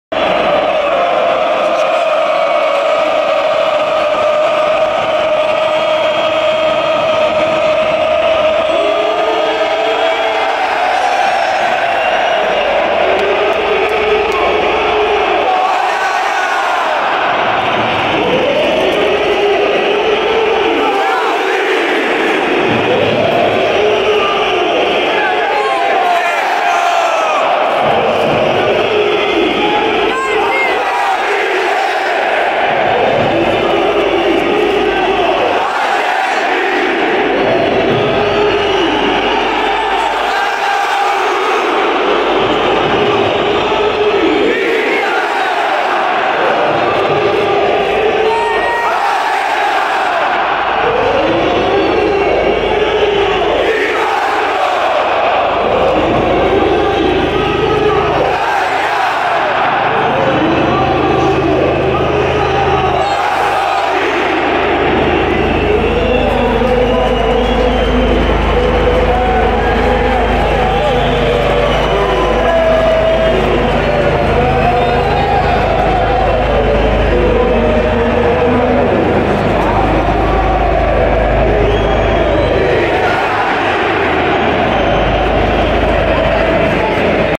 [4K] Inter - Milan, COREOGRAFIA + FORMAZIONE INTER 🖤💙
La pazzesca coreografia di tutto lo stadio con annessa lettura della formazione nerazzurra.